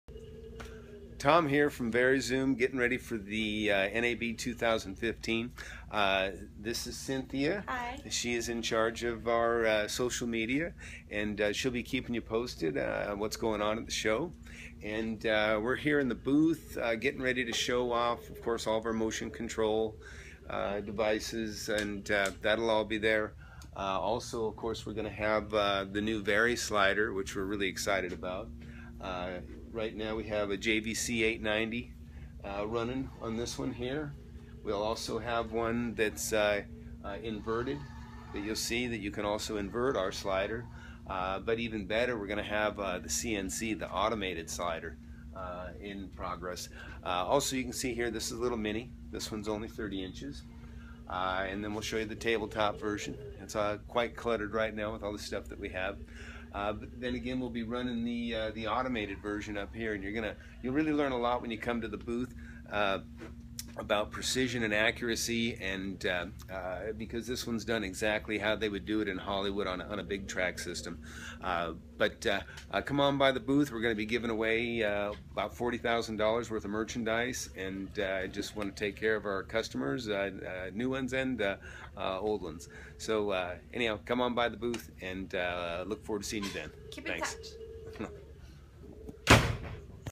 NAB 2015 PreShow VariSliders in VariZoom booth Preparation
Quick peak at our VariSlider and motion control display for NAB 2015 we have what we think will be the best camera slider system available. Manual and fully automated with the same type of academy award winning engineering we have all seen in custom one of a kind movie rigs used on on major motion pictures. Frame accurate repeat pass technology built to last and pass to the next generation.